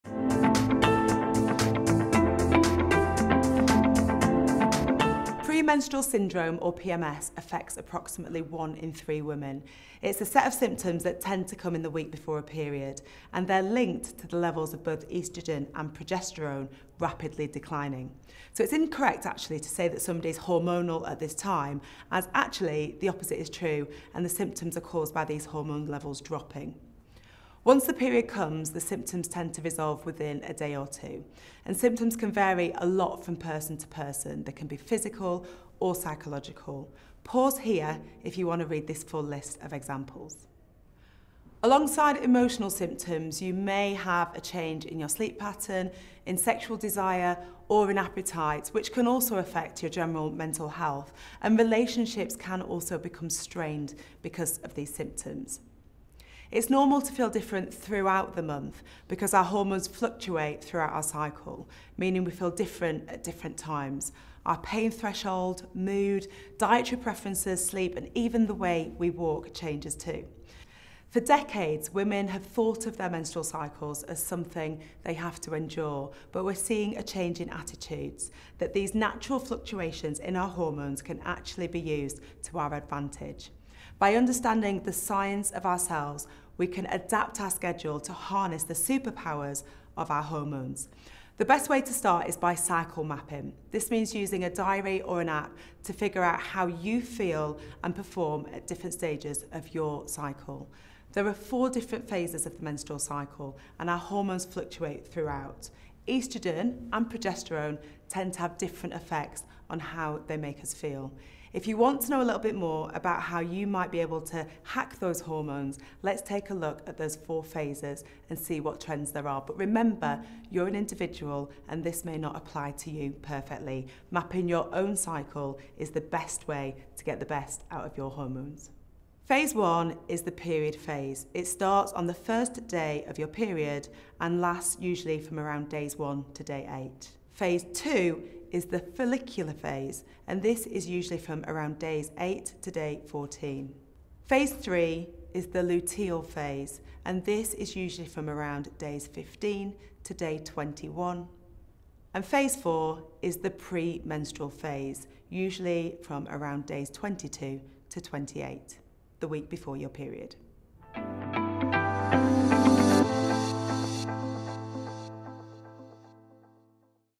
Bupa | Inside Health | Women's Health | PMS - am I normal?
PMS (Premenstrual syndrome) can make you feel moody, bloated, and achy. But what exactly is PMS and why does it happen? Dr Zoe Williams is here to explain the science behind this common condition and how you can cope with it better.